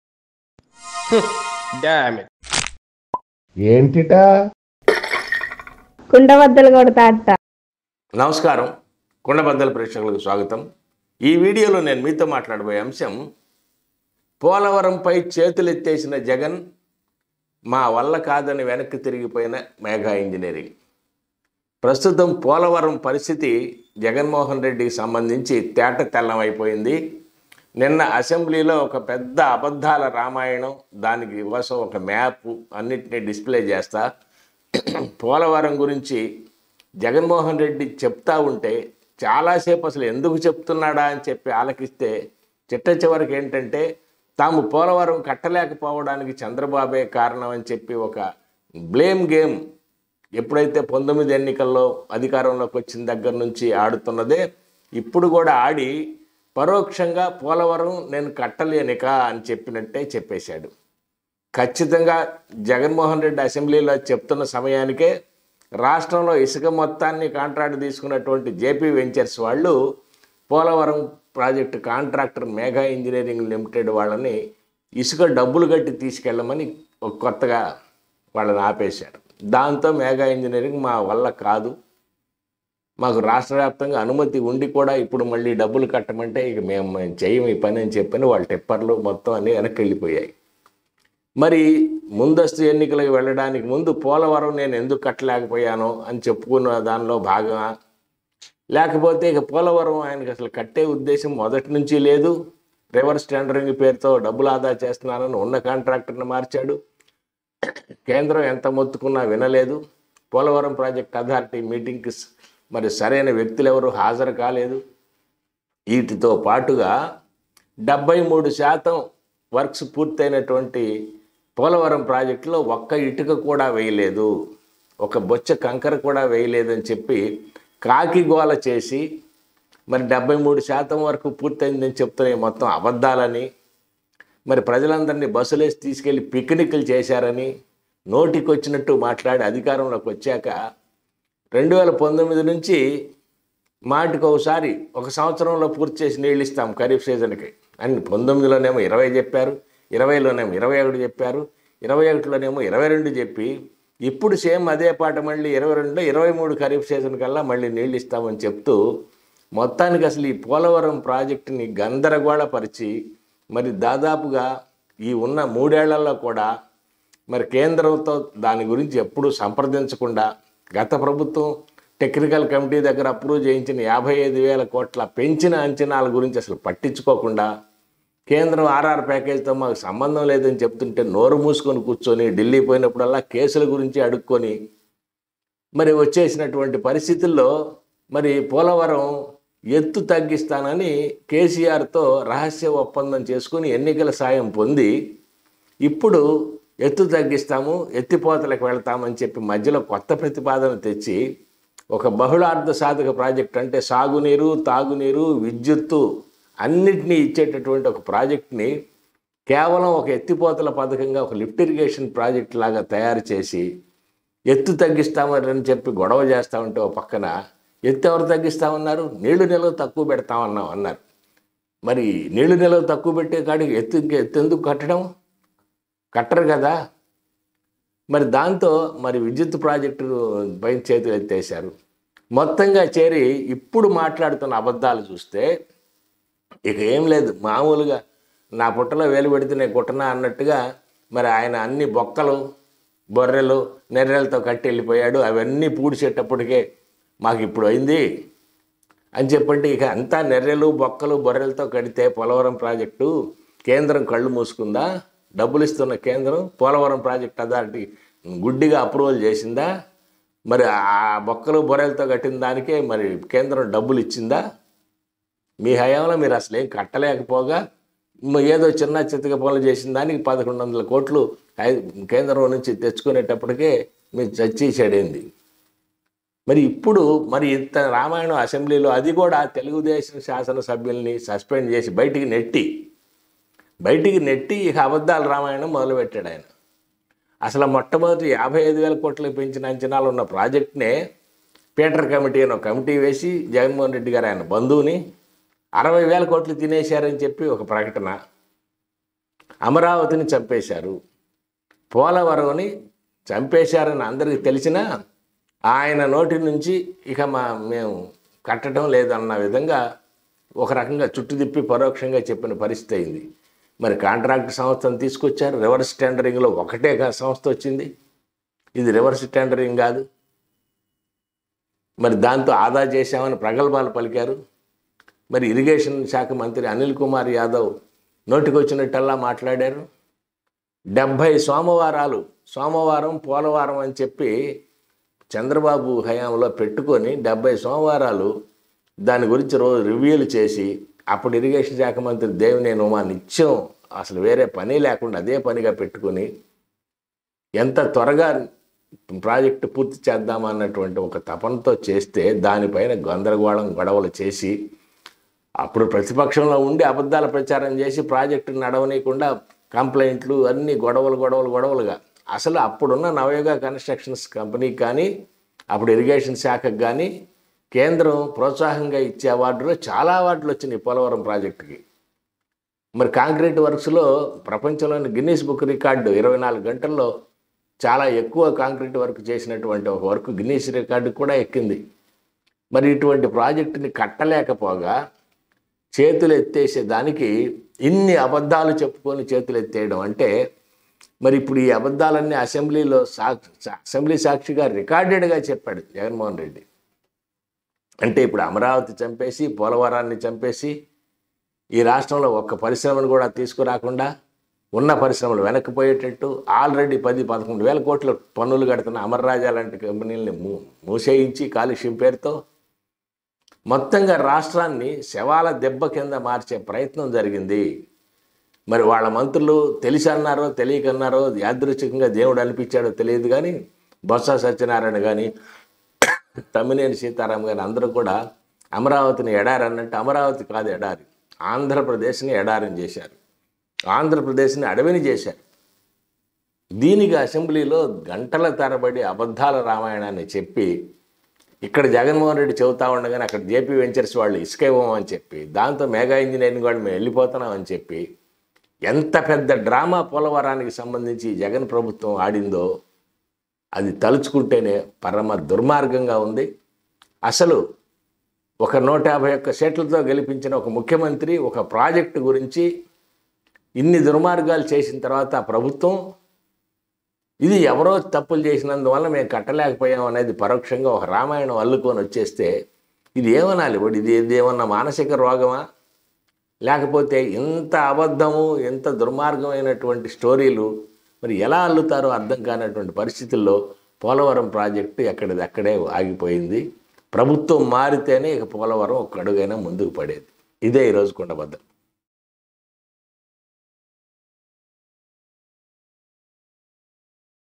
పోలవరం పై చేతులు ఎత్తేసిన జగన్ | వెనక్కి మళ్లిన కాంట్రాక్టర్ | YS Jagan | MEGA
Chandrababu Naidu,
Kundabaddalu Subbarao,
Devineni Uma,
Polavaram Project,